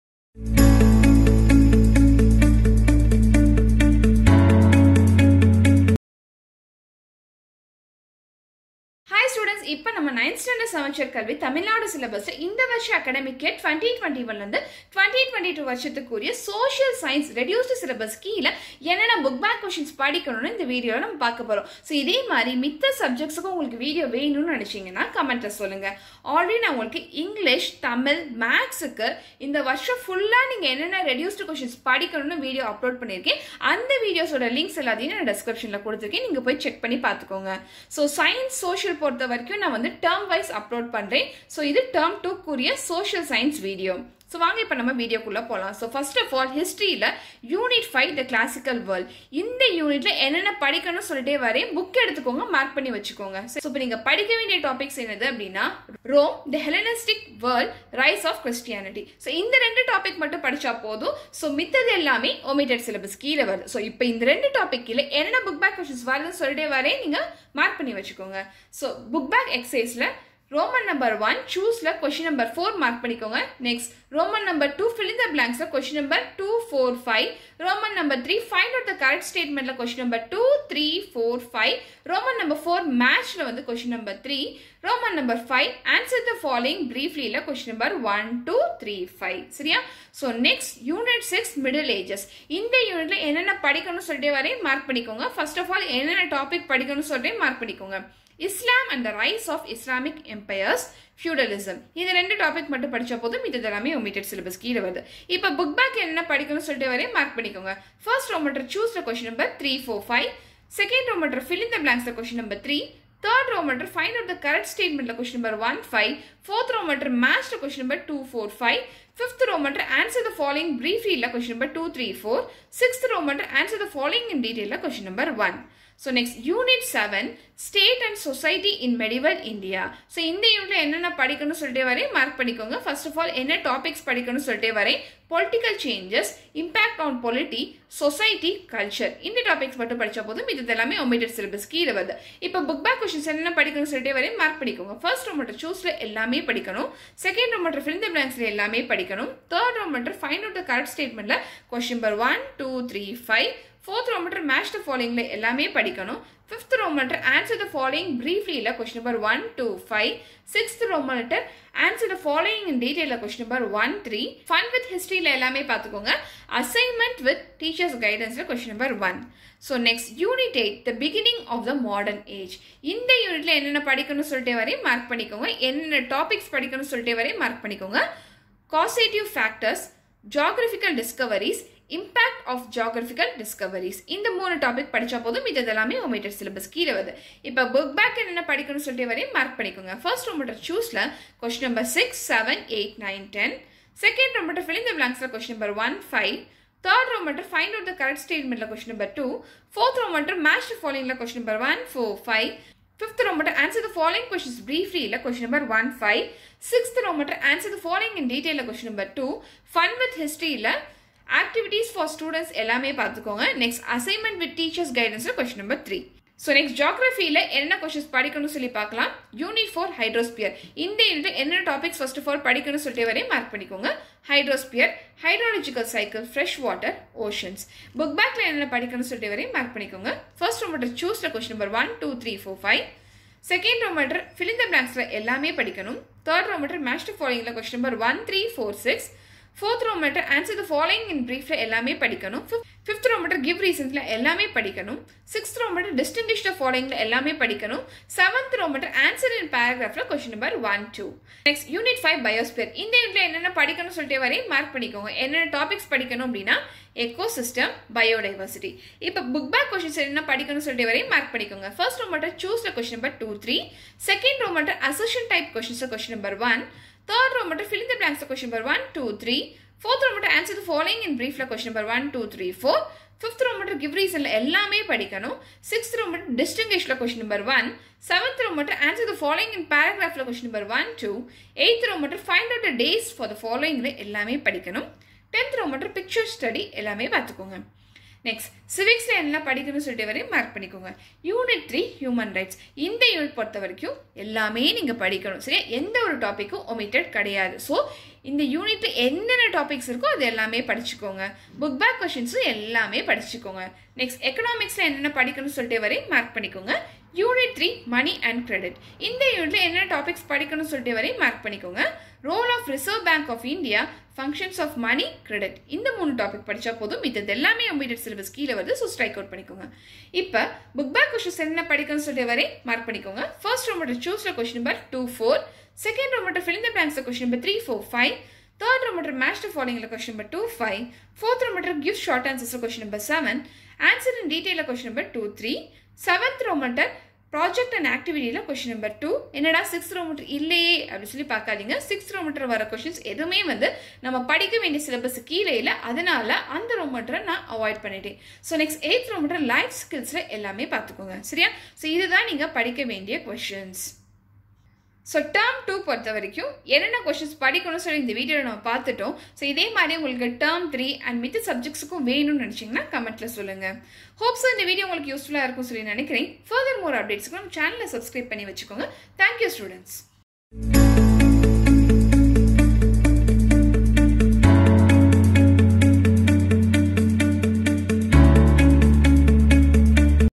Tamilnadu Samacheer 9th Std Socialscience Bookback questions for Reducedsyllabus (2021-22) 2nd Term
Hi students,
This video is about reduced bookback portion for 9th Std Social (Tamilnadu syllabus) for Academic year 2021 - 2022.
9th std samacheer syllabus
social(Reduced syllabus)